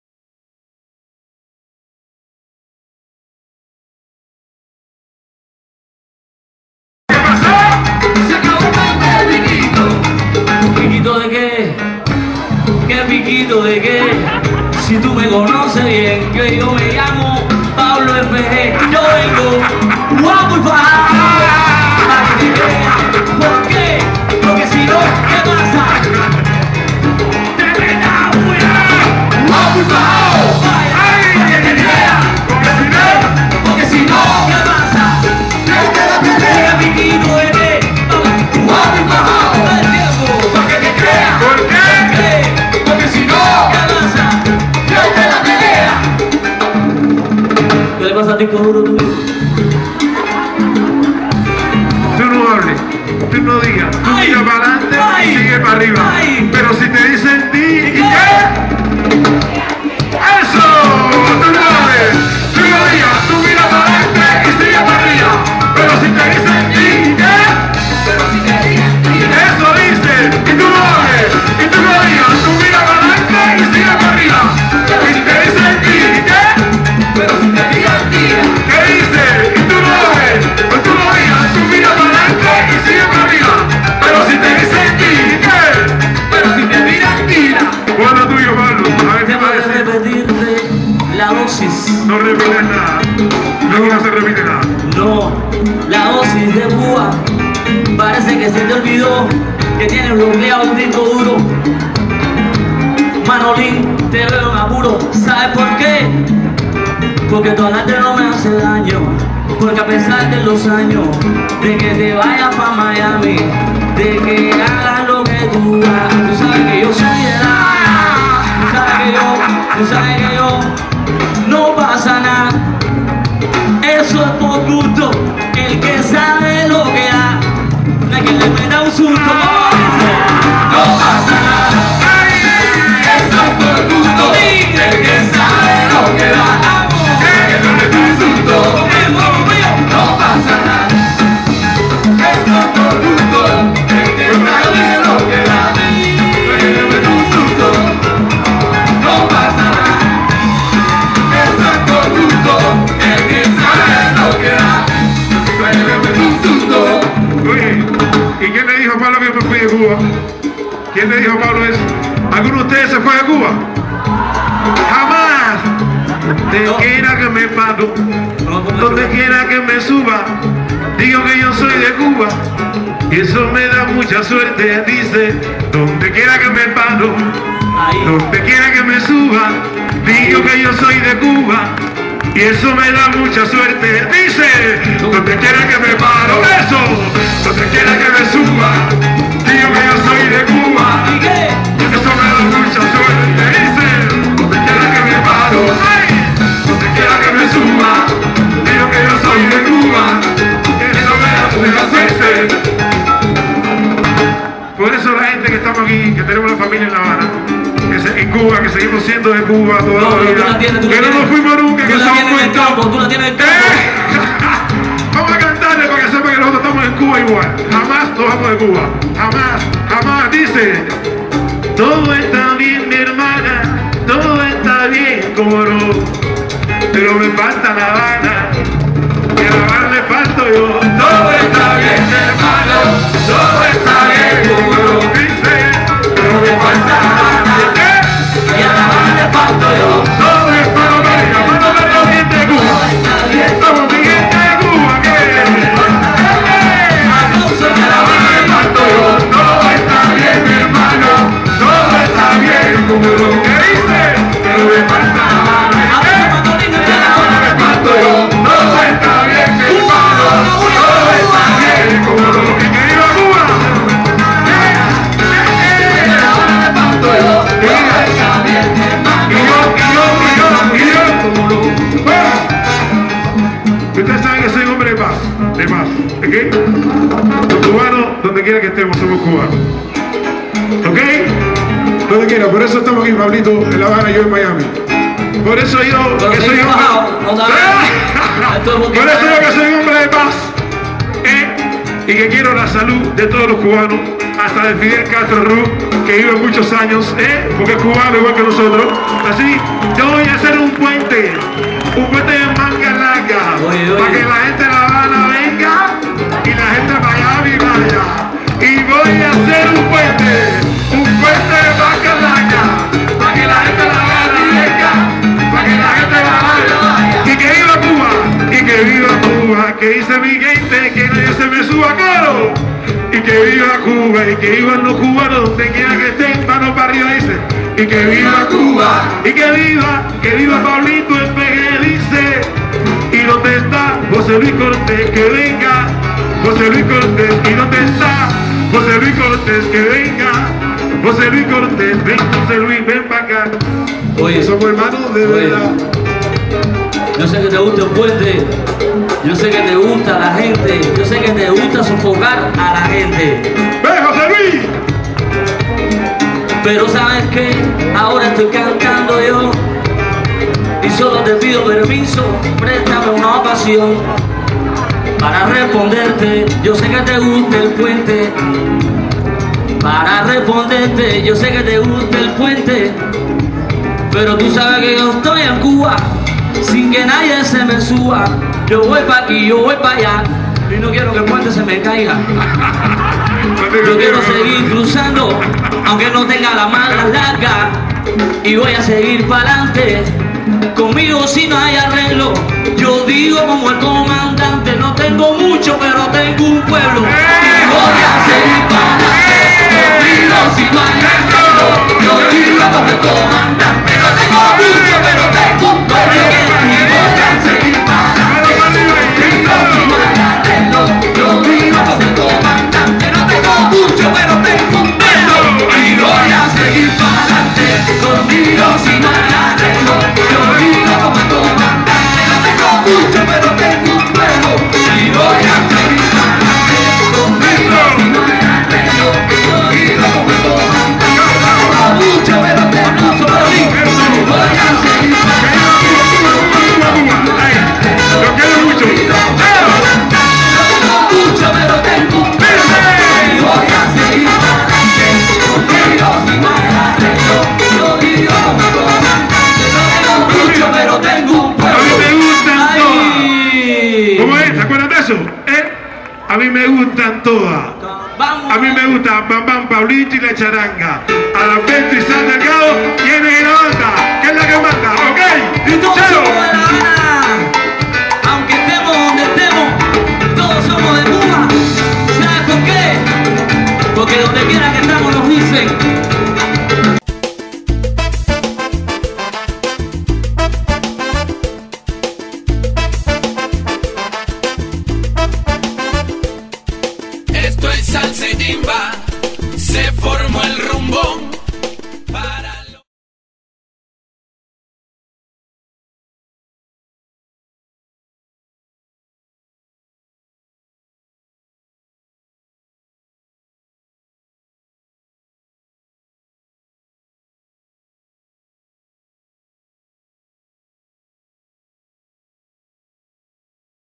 Paulo Fg y Manolin Mano a Mano Live 2DA PARTE
Paulo Fg y Manolin Mano a Mano Live 2DA PARTE. dos grandes Paulo Fg El Sofocador de la Habana y Manolin El Medico de la Salsa, un mano a mano...A LA BATALLAAA ..DALE PLAYY¡¡¡  …DE SALSA Y TIMBA ..MISELLLO..QUE NO HAY MAS NAAAAA¡¡¡¡